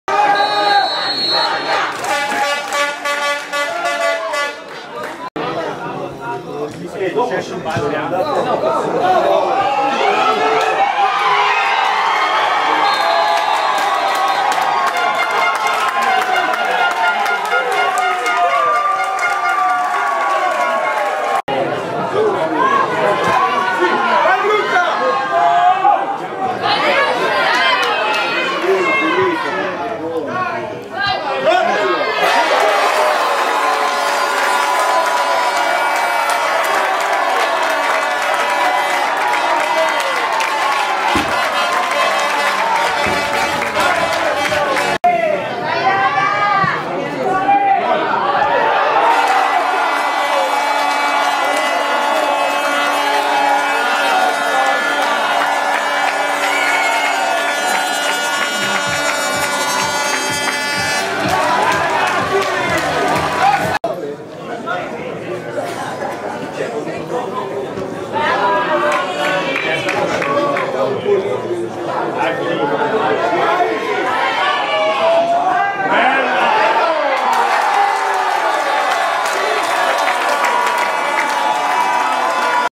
GIOVANISSIMI NAZIONALI, ANDATA OTTAVI - Novara-Sassuolo 3-1, la sintesi
Guarda la sintesi della gara di andata degli ottavi di finale Scudetto tra Novara e Sassuolo. Gli Azzurrini, dopo esser andati sotto al 3', hanno recuperato grazie alle reti di Beretta, Costa e Stoppa. Domenica il ritorno sarà decisivo per l'accesso alle Final Eight.